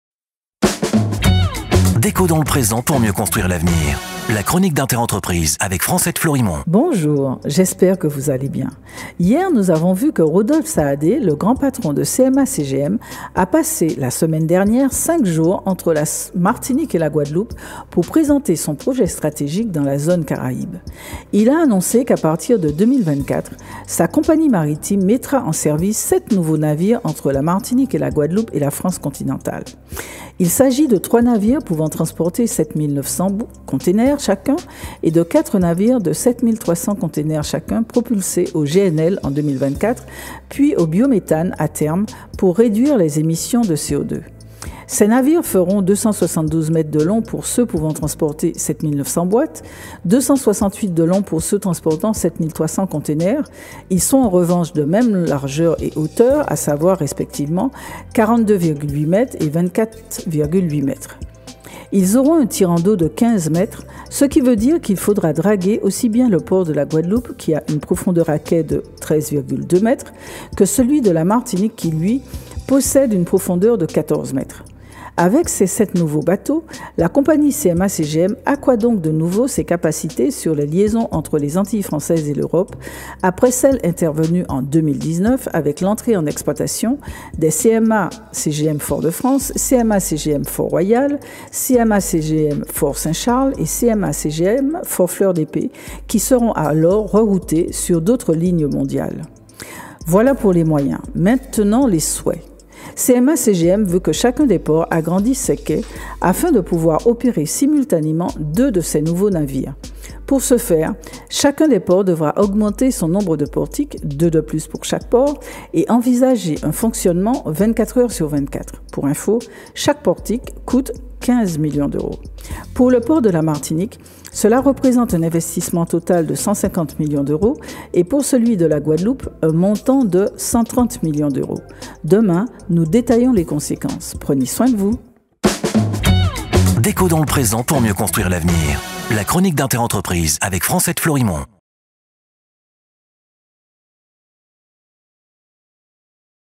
DLPPMCA • CMA CGM : Rodolphe Saadé en Martinique, puis en Guadeloupe - Partie 2
7 nouveaux navires et 300M€ d'investissement à réaliser par les ports pour les recevoir